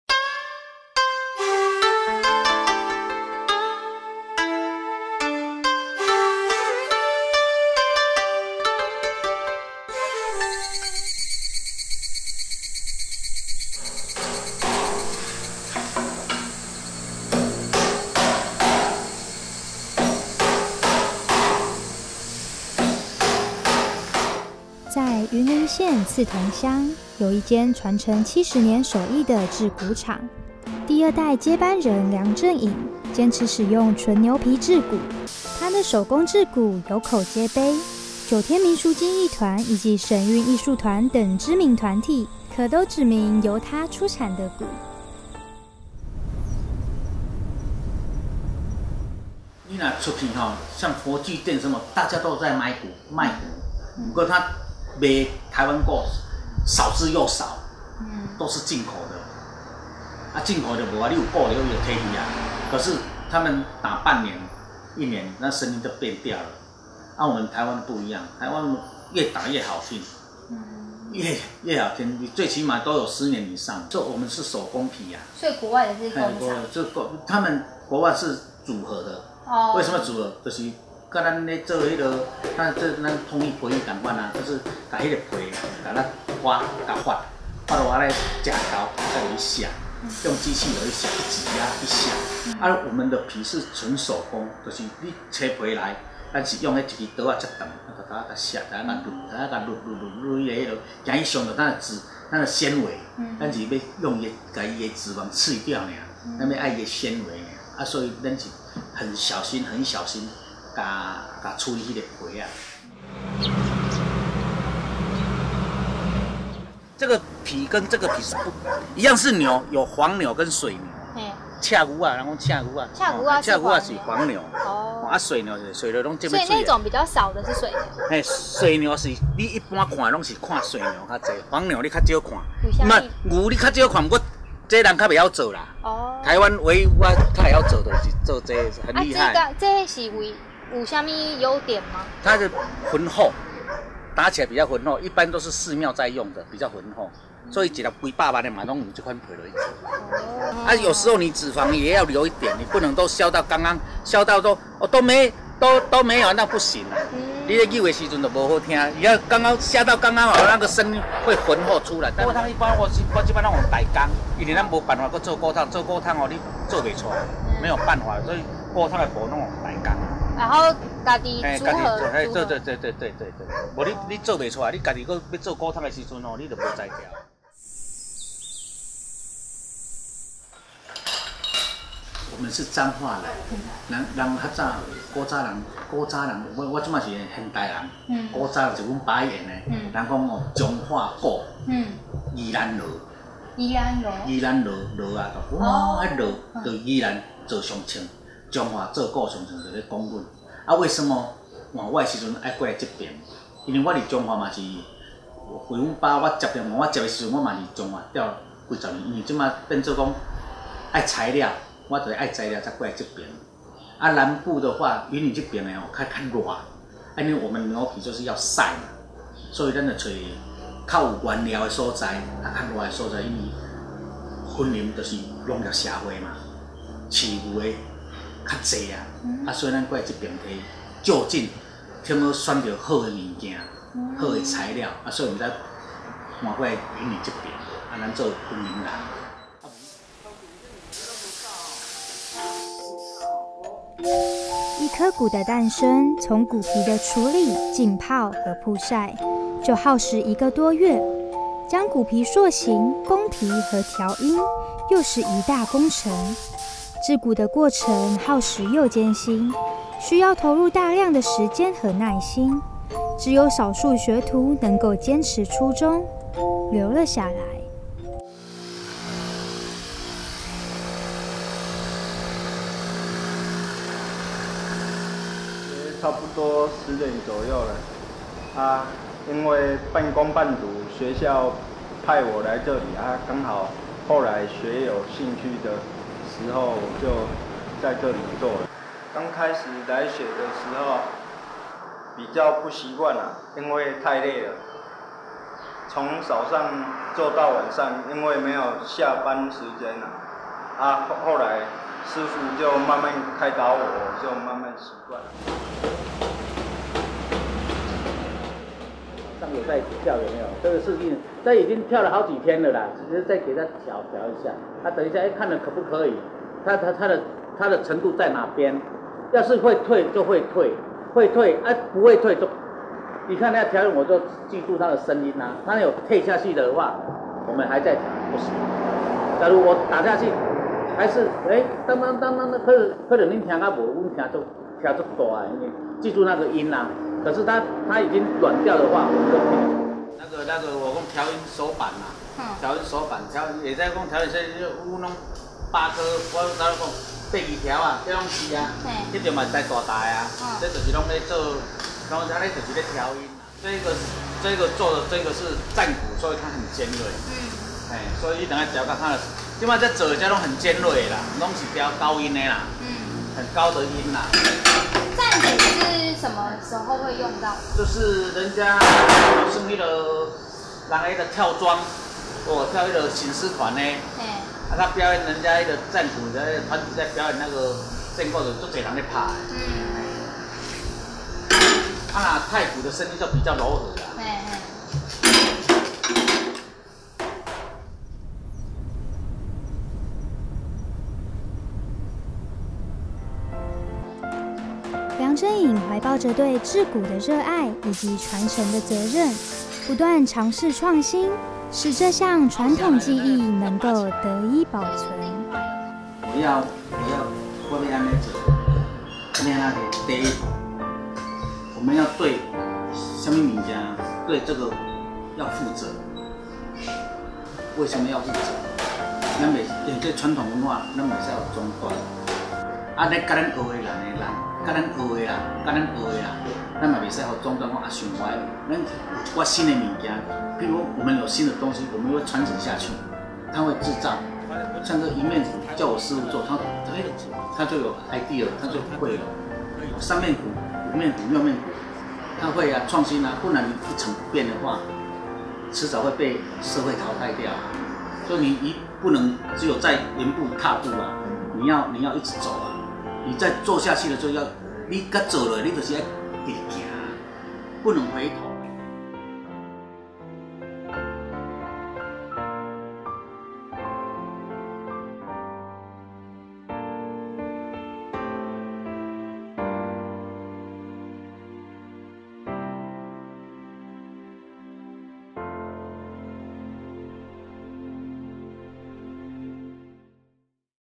製鼓人
【記者　陳禹豪、吳香穎／雲林莿桐報導】

片長：7分43秒

不論是廟宇慶典或是陣頭和八家將表演，鼓的演奏都是不可或缺的重要角色。在雲林縣莿桐鄉，有一間手工製鼓廠，老師傅的技藝遠近馳名，是受許多表演團體青睞的傳統手工鼓。從鼓皮的處理到鼓的調音，都是由師傅親自操刀，每一顆鼓可都是為表演者量身訂做。